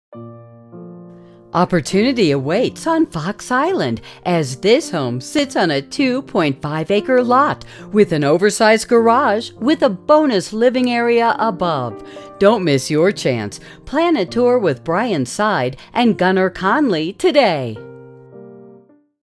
702 9th Avenue Fox Island, WA
702 9th Avenue, Fox Island, WA

You won’t find a better opportunity on Fox Island. This home sits on approximately two and one-half acres and offers large living spaces with an easy flow between the living/dining areas. Enjoy an oversized garage with a BONUS 560 square-foot newly finished living area above for a home office or sep